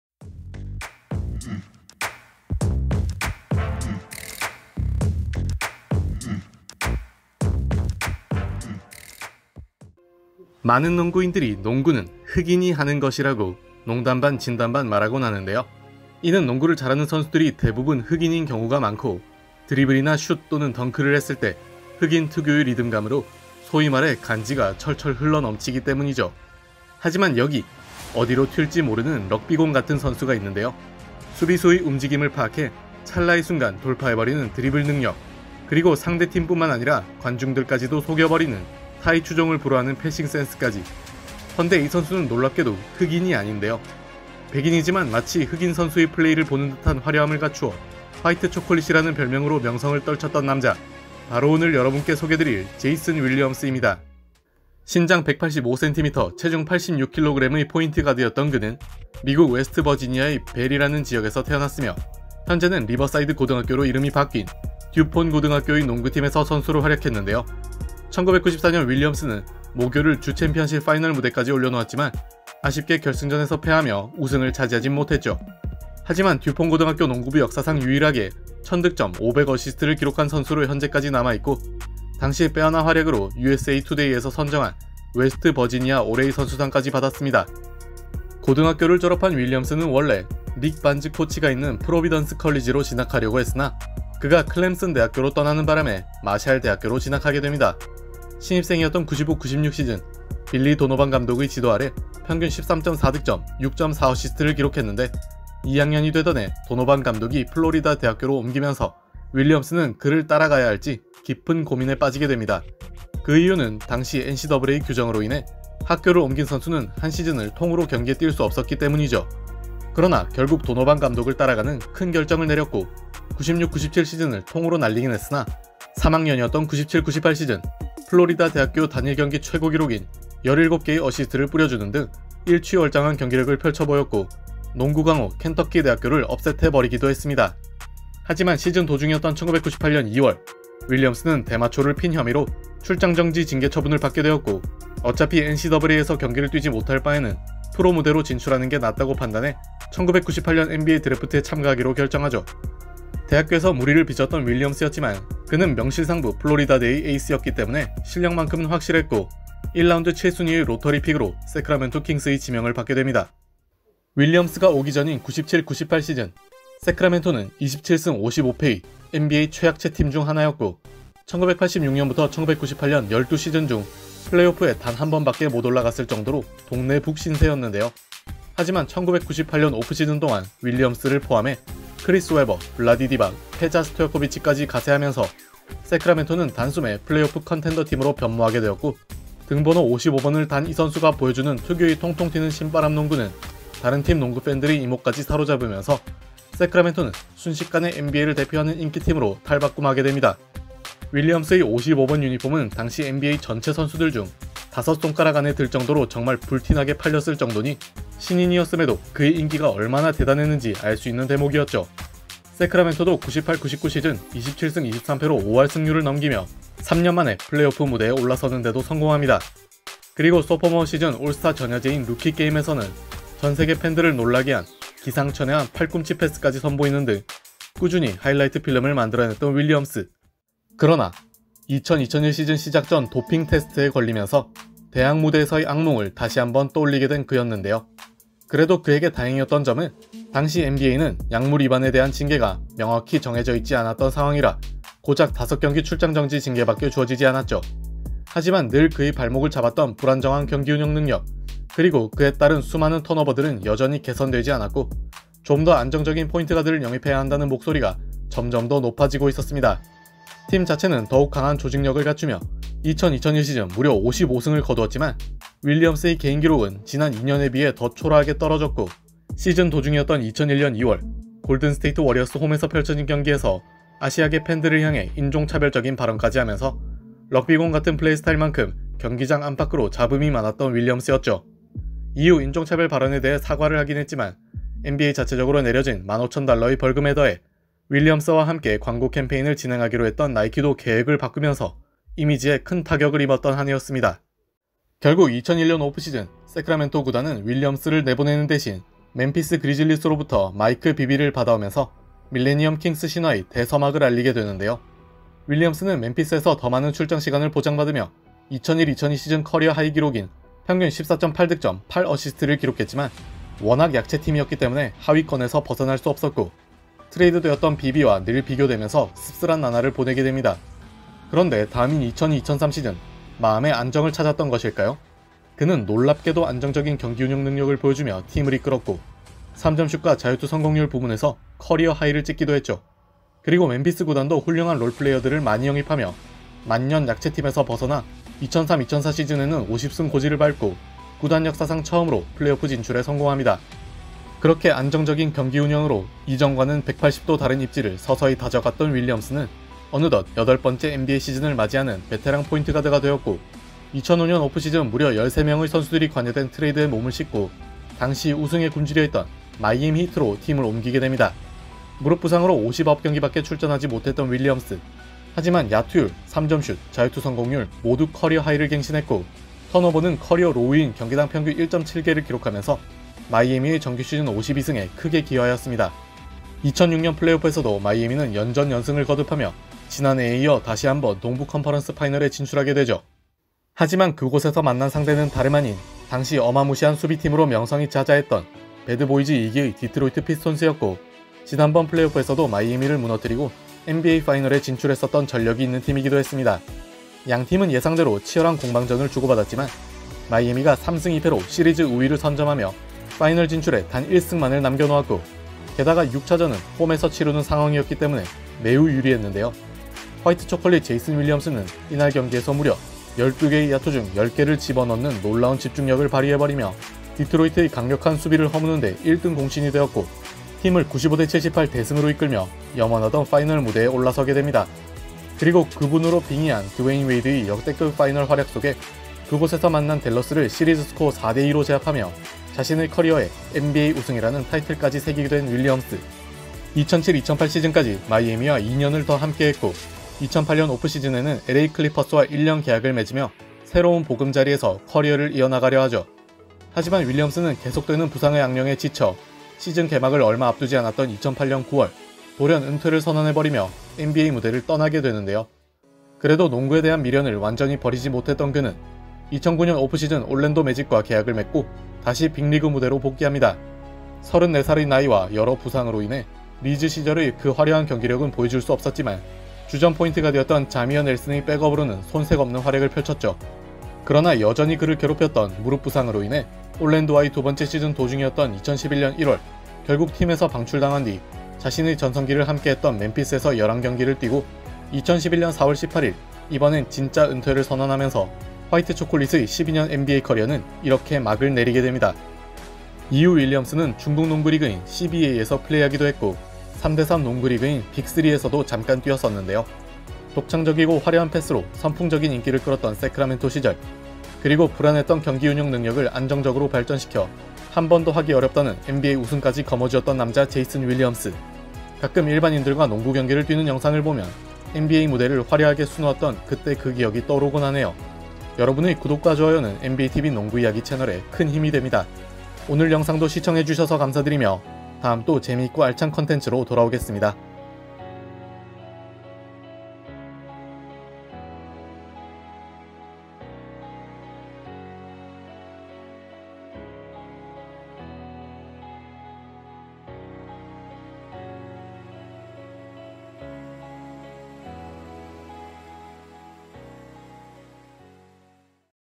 흑인의 소울을 가졌던 백인 포인트가드 "제이슨 윌리엄스" 스토리! 😲
만년 약체 새크라멘토 킹스를 단숨에 인기팀으로 만들어버렸던 루키!
"화이트 초콜릿" 제이슨 윌리엄스 😎